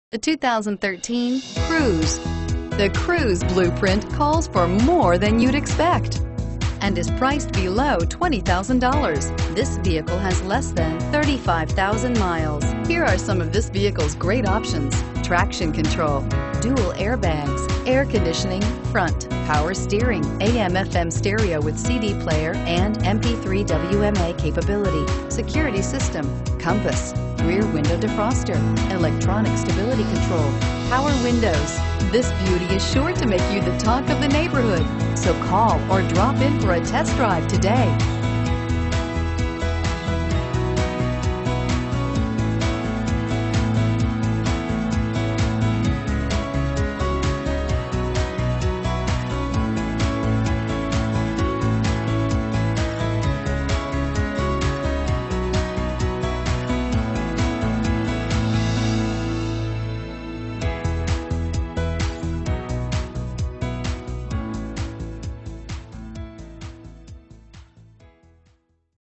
2013 Chevrolet Cruze Ankeny Des Moines Ames Iowa 14D817
Turbo! Isn't it time for a Chevrolet?! Type your sentence here. Looking for an amazing value on a wonderful 2013 Chevrolet Cruze? Well, this is IT! Want to save some money? Get the NEW look for the used price on this one owner vehicle. Previous owner purchased it brand new and it still looks like the day it rolled off the lot! to view a Free Price Check!